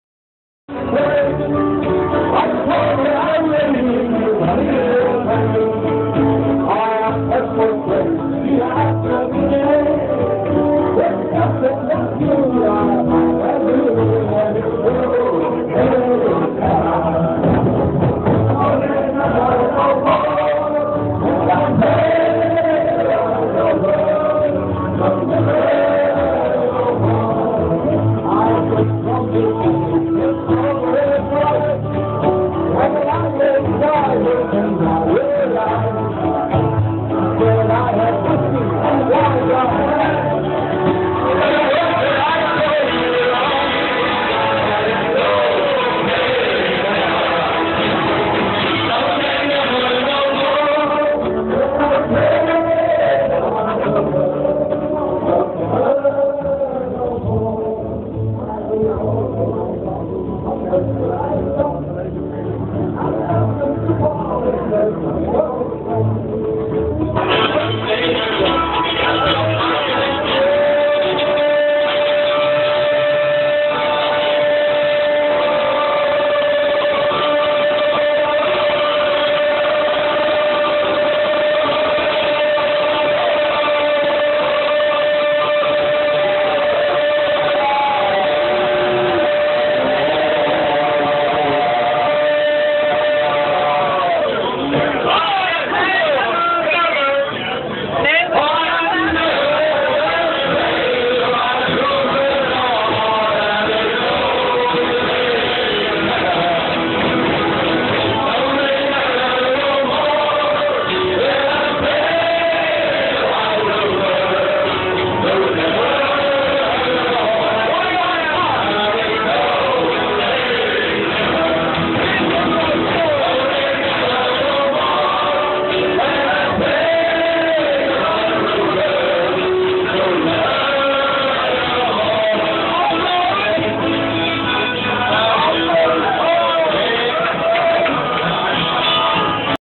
Aboard the Étoile du Roy at the Oostende Voor Anker 2015 Maritime Festival.
The Historic Ships Council UK crew of the Spencer enjoying the atmosphere of the Captain and Crew Party aboard the wonderful Tall Ship Étoile du Roy during Oostende Voor Anker 2015 Maritime Festival.